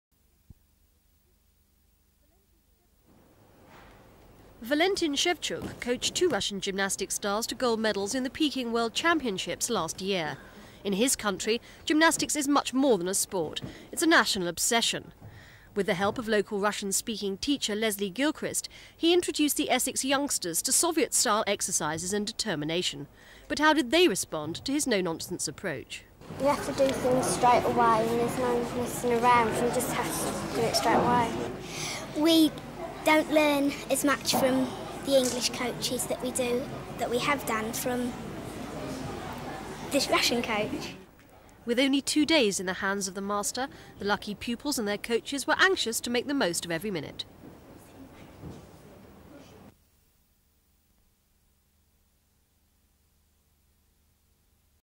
Essex Gymnasts Coached by Medal Man Valentin Shevchuk | Thames News
Russian star Valentin Shevchuk coached two Russian gymnasts to World Championship gold medals and has now introduced young Essex gymnasts to Soviet-style excercises and a no-nonsense approach.